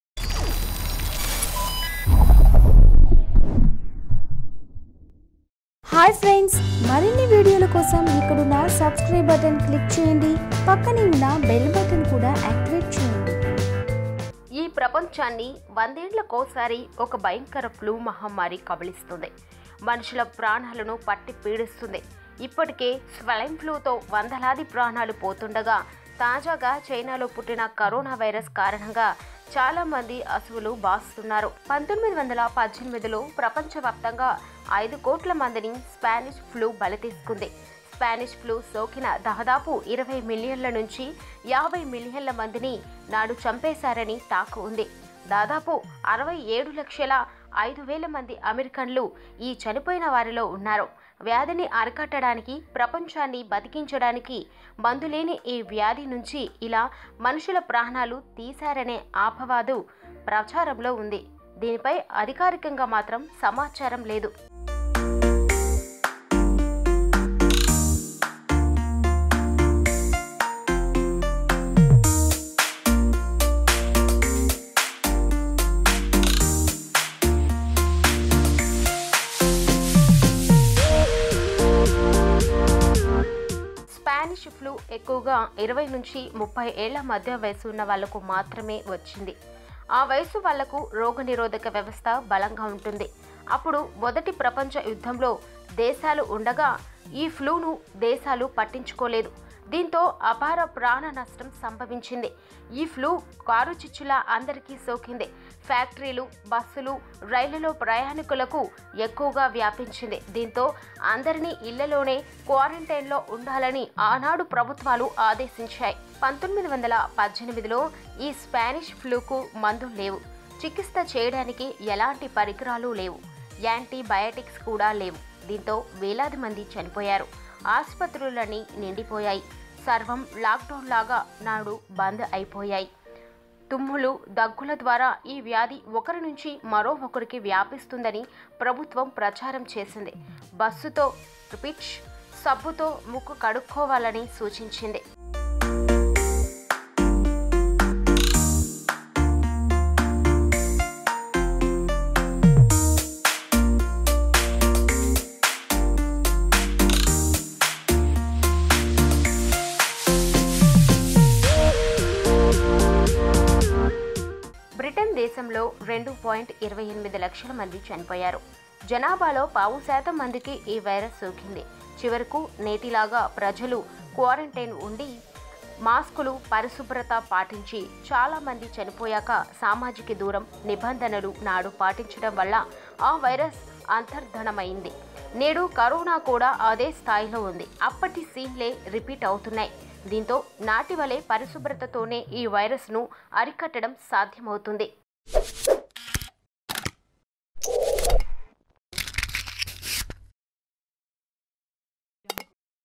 5 కోట్ల మందిని చంపిన స్పానిష్ ఫ్లూ ఎలా తగ్గింది? ||VS9NEWS
Watch:
5 కోట్ల మందిని చంపిన స్పానిష్ ఫ్లూ ఎలా తగ్గింది?

ఈ ప్రపంచాన్ని వందేండ్లకోసారి ఒక భయంకర ఫ్లూ మహమ్మారి కబళిస్తోంది. మనుషుల ప్రాణాలను పట్టిపీడిస్తోంది. ఇప్పటికే స్వైన్ ఫ్లూతో వందలాది ప్రాణాలు పోతుండగా.. తాజాగా చైనాలో పుట్టిన కరోనా వైరస్ కారణంగా చాలా మంది అసువులు బాస్తున్నారు.